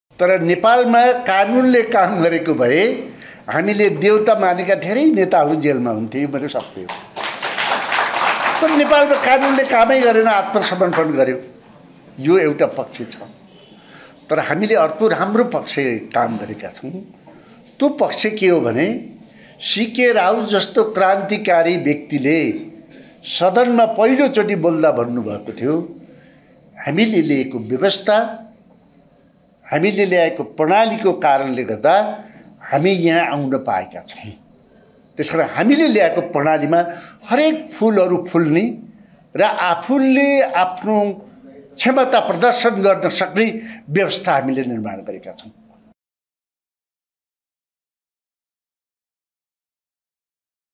कानुनले काम गरेको भए जनताले देउता मानेका व्यक्तिहरु जेलमा हुन्थे : चन्द्र भण्डारी | Dainik Nepal
कानुनले काम गरेको भए जनताले देउता मानेका व्यक्तिहरु जेलमा हुन्थे : चन्द्र भण्डारी